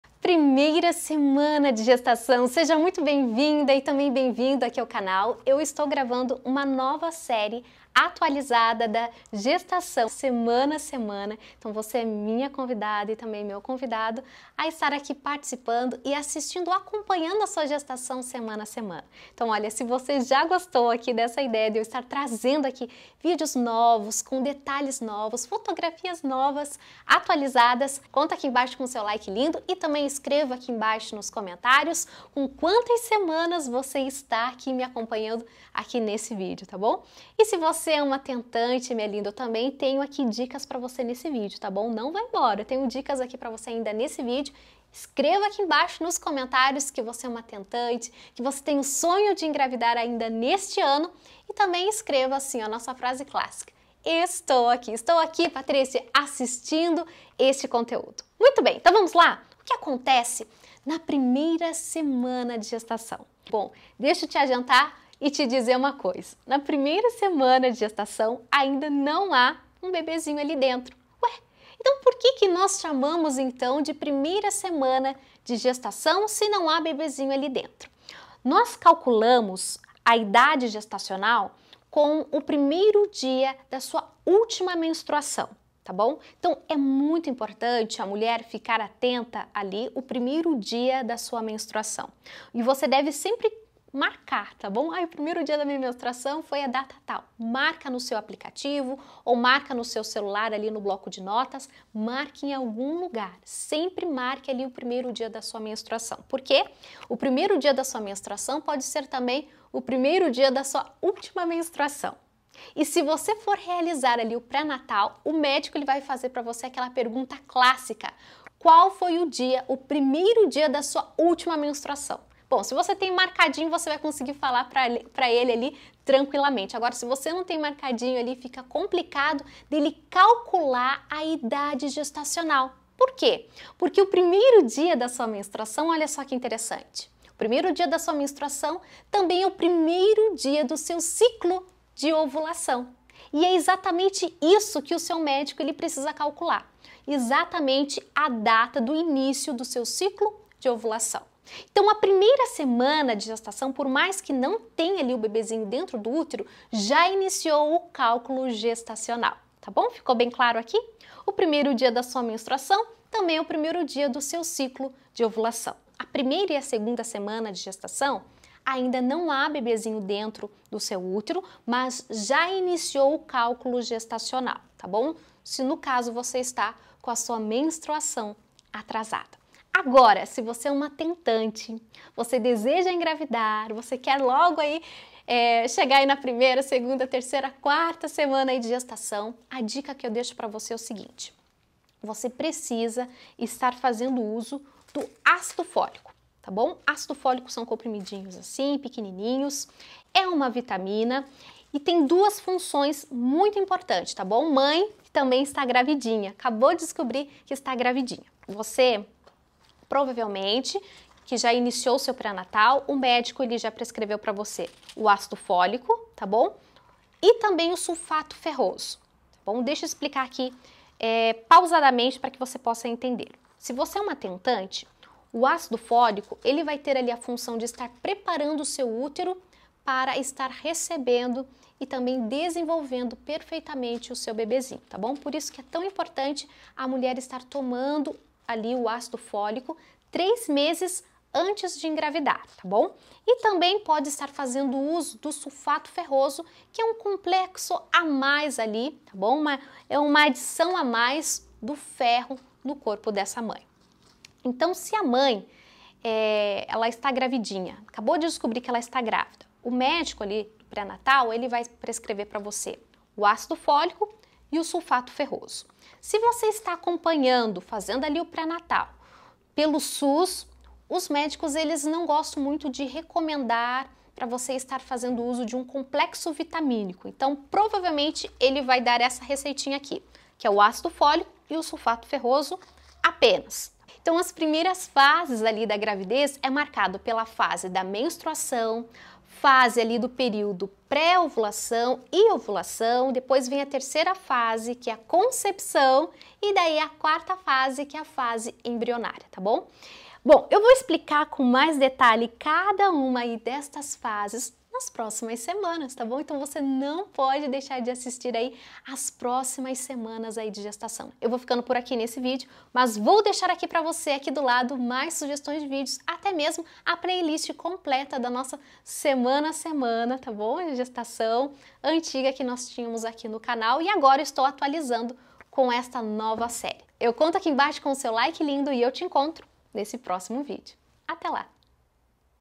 1ª SEMANA DE GRAVIDEZ - Gravidez por semana | Patricia Moreira
Na primeira semana de gravidez ainda não há bebê no útero, mas a contagem gestacional já iniciou. Assista ao vídeo e saiba o que esta acontecendo em seu útero neste momento.